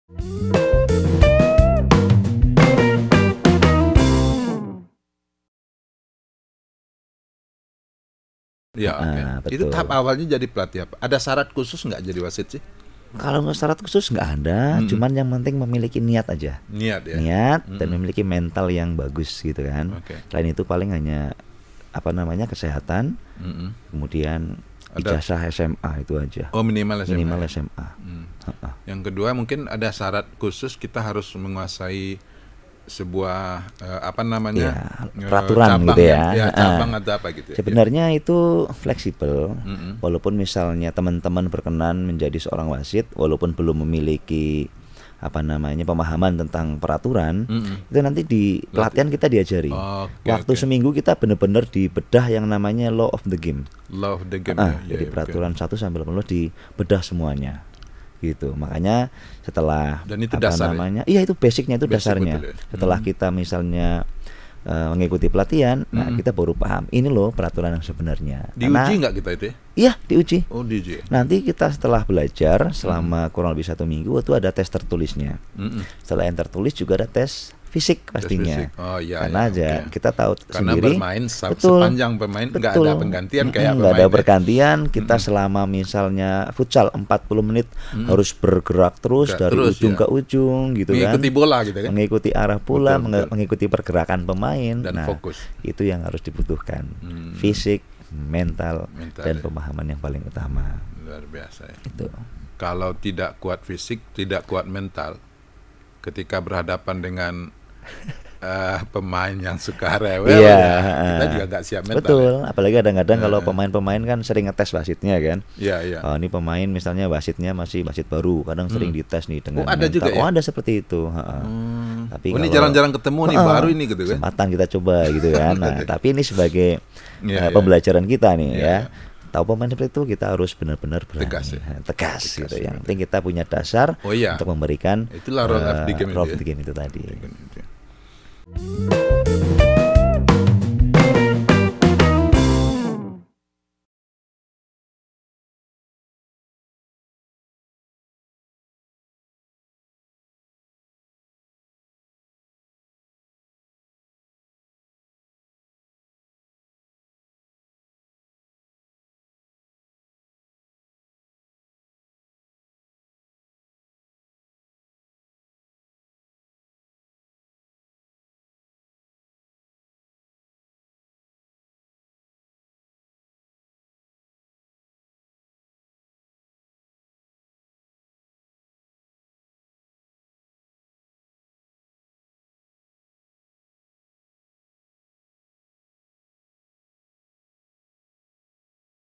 Mau Jadi Wasit Ini Syarat nya - Tripod Sport
Sebuah pertandingan tidak bisa berjalan tanpa wasit. Namun, wasit kerap menjadi sasaran protes pemain dalam sebuah pertandingan, seperti sering terlihat di cabang sepak bola dan futsal.

Bagaimana wasit menghadapi tekanan ketika menjadi wasit? Kenapa ada orang mau jadi wasit? Apakah wasit sudah diperlakukan layak di sebuah pertandingan